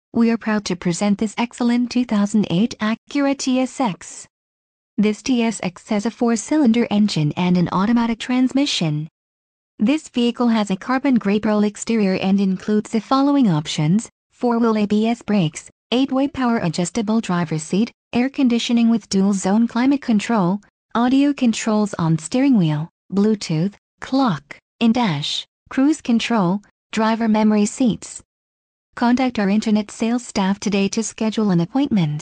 2008 Acura TSX Acura by Executive2
Acura by Executive2

2008 Acura TSX
Engine: 2.4L 4 cyls
Trans: Automatic 5-Speed
Color: Carbon Gray Pearl
Mileage: 33,078